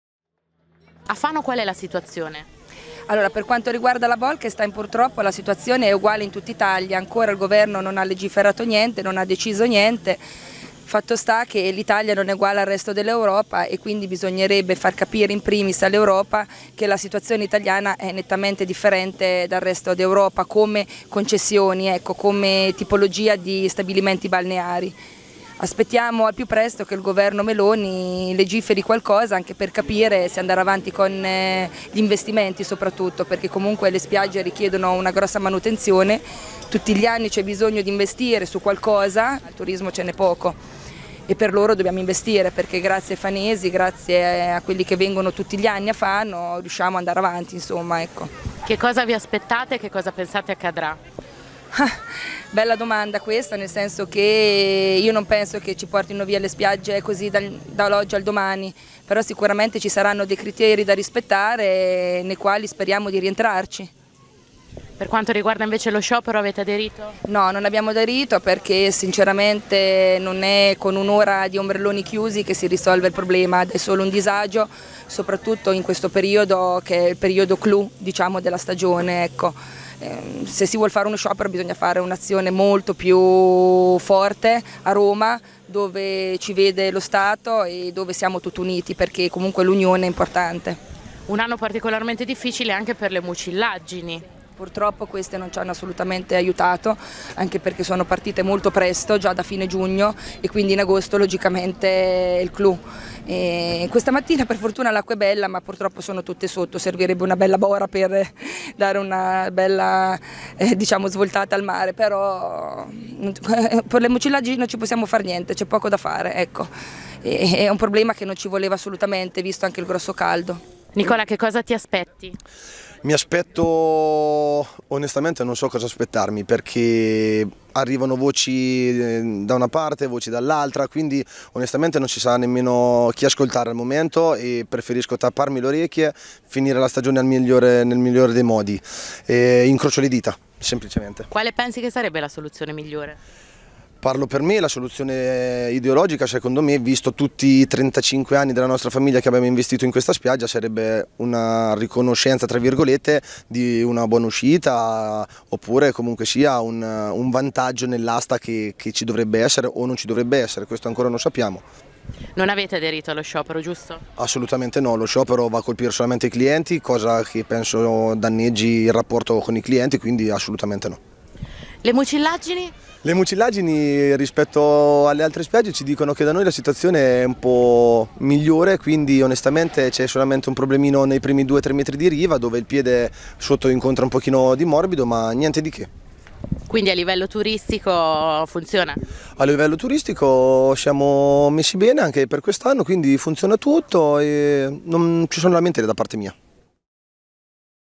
Legge Bolkestein, spiagge Fano: “Sciopero inutile”
A Fano i gestori degli stabilimenti balneari hanno deciso di tenere aperti gli ombrelloni, definendo inutile questo tipo di protesta che andrebbe solo a discapito dei clienti.